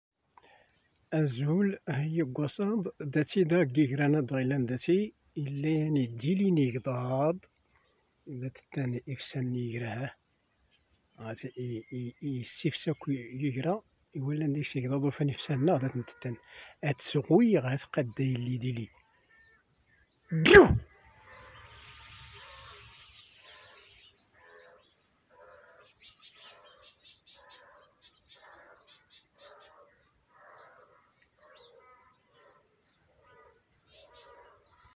Idili n igḍaḍ ! 17 août 2022.
Une nuée des passereaux et les braiement des ânes à Wassade !!
Idili n igḍaḍ d ishuṛṛan n iɣwyal g Waṣaḍ !!